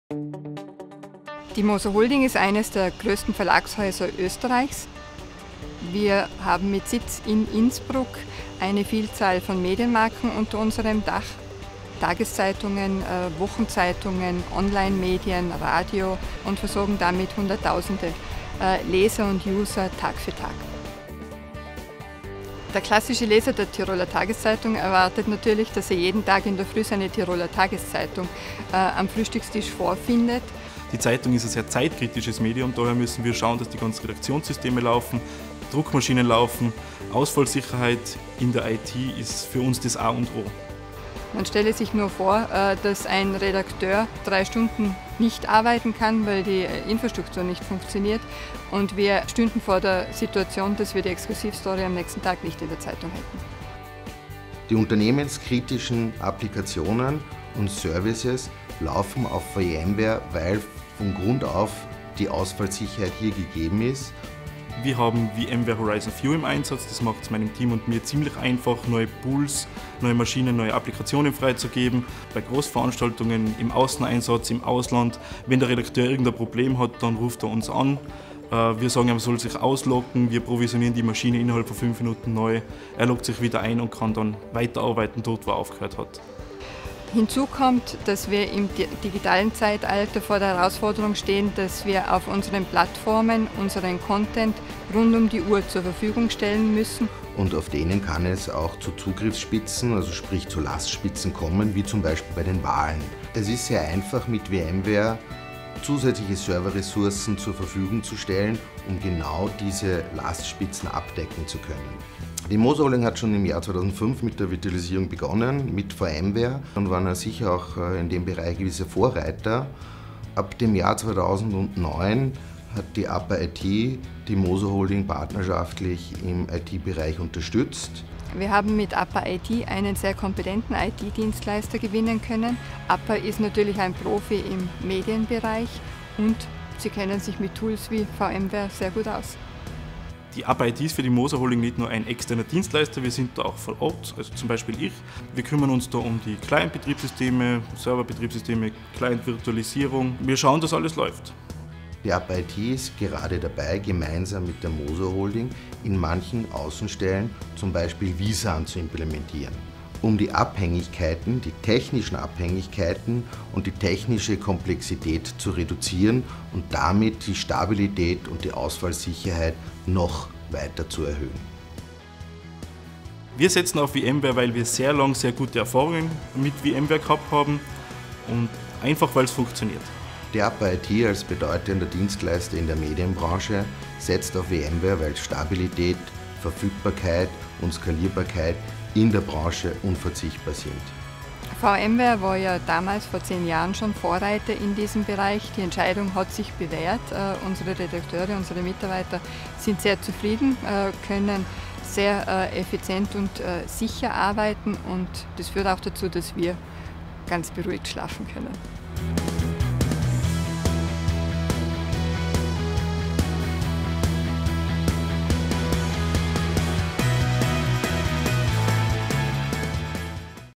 24/7 Echtzeit-Berichterstattung dank Virtualisierung
Für das viertgrößte Medienunternehmen Österreichs ist Ausfallsicherheit ein Muss, um weiterhin im kompetitiven Umfeld bestehen zu können.